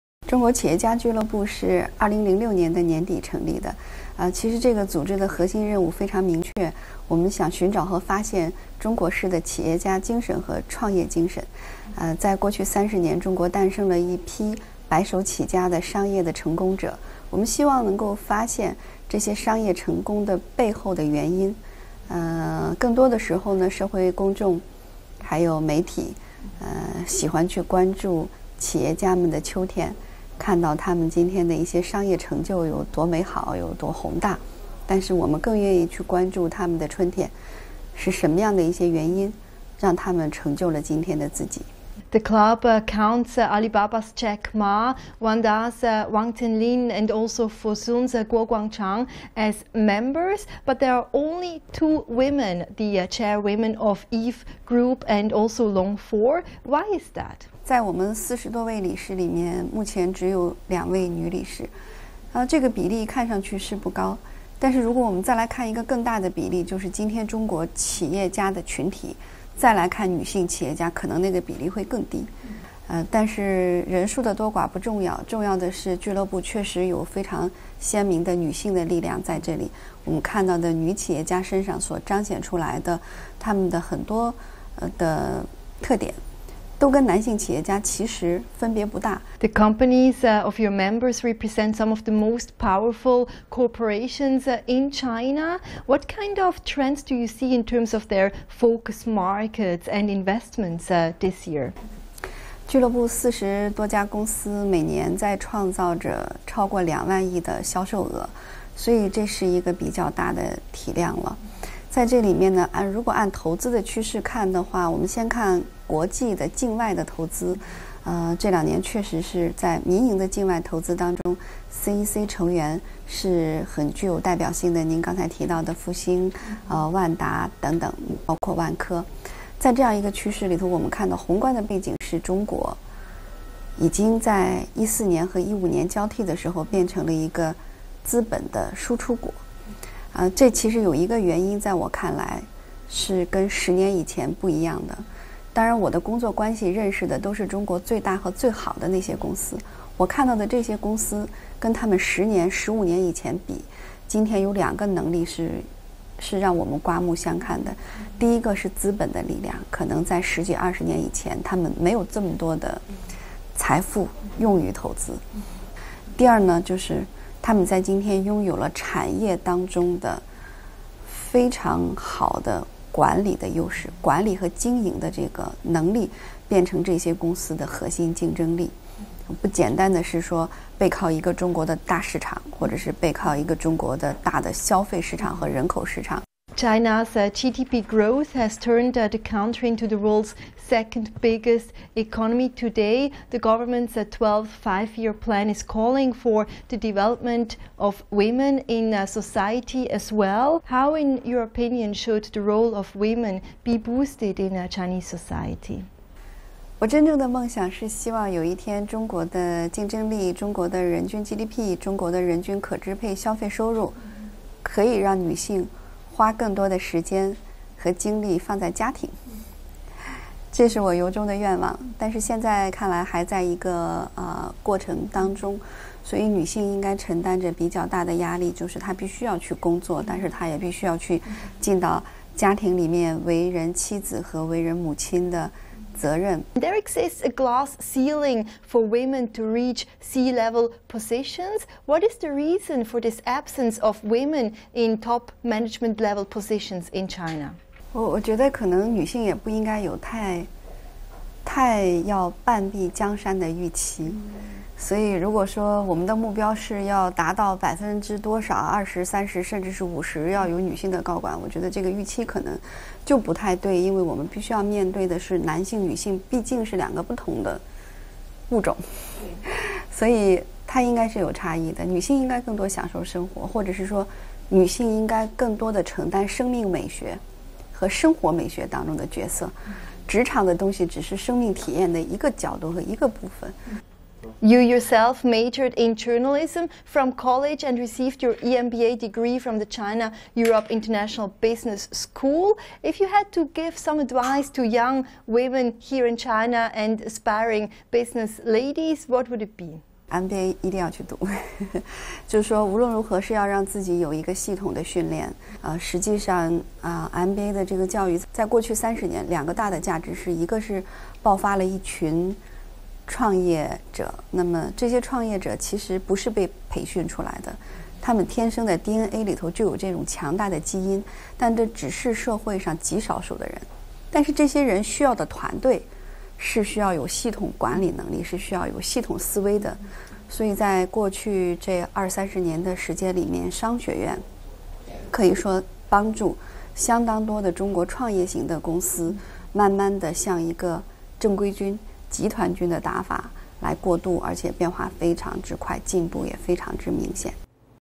Female head of a China's billionaires club one-on-one with CCTV
Cheng Hong - or Maggie Cheng as she's known, heads up the prestigious China Entrepreneur Club - an exclusive club of billionaires which counts Alibaba's Jack Ma, Wanda's Wang Jianlin, Fosun's Guo Guangchang, and the likes as its members. CCTV’s Martina Fuchs sat down with her to find out why there are so few women in senior management and C-level positions in China, and what business ladies can do to shake up the corporate world here.